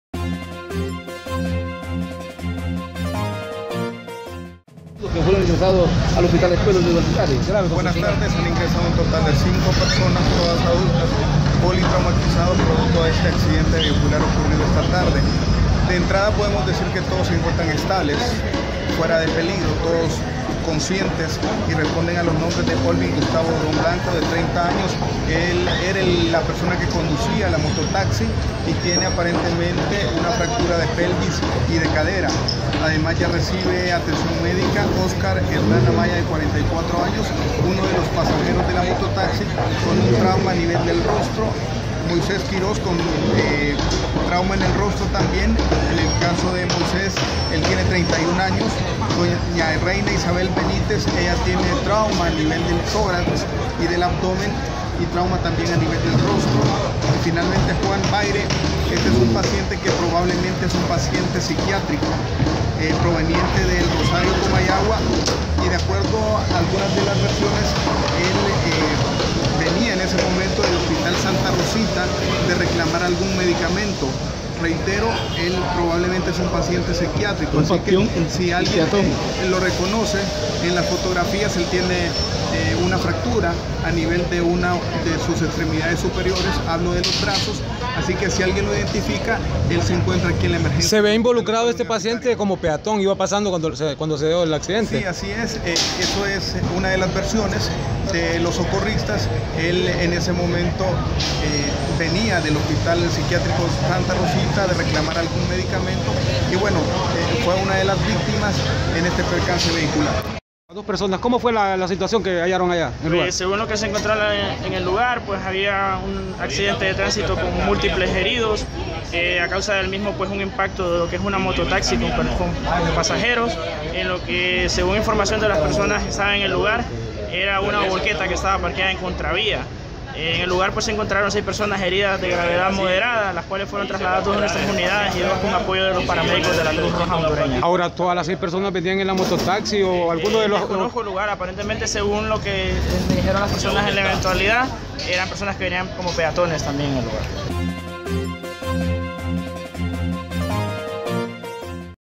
Diario La Tribuna (Honduras)
Cinco heridos deja accidente entre mototaxi y volqueta